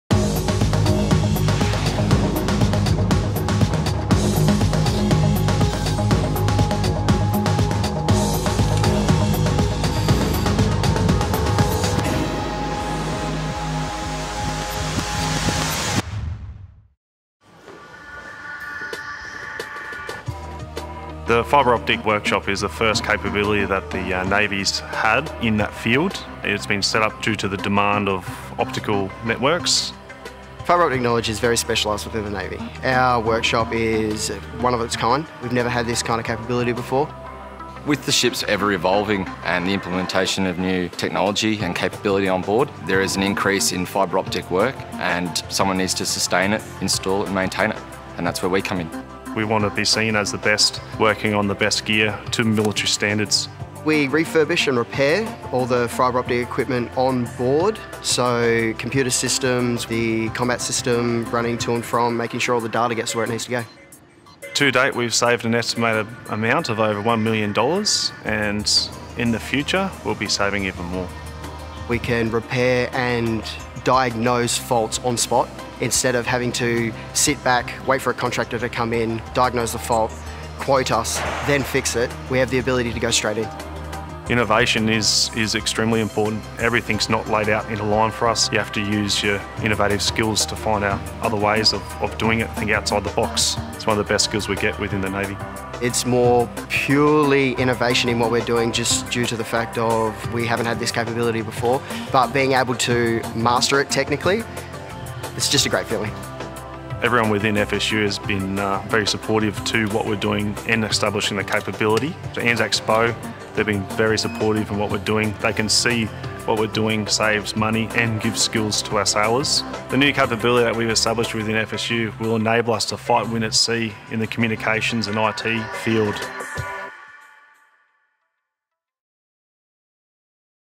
Naval Engineering - Fibre Optic Cable Maintenance and Repair
Fibre optics are widely used throughout the RAN platforms and quite especially specialised within the Navy. Sailors at the Fibre Optic workshop at Fleet Support Unit in Western Australia talk about their work and how they have improved optical fibre technical support, installation and repair capability within the Royal Australian Navy, saving money and time.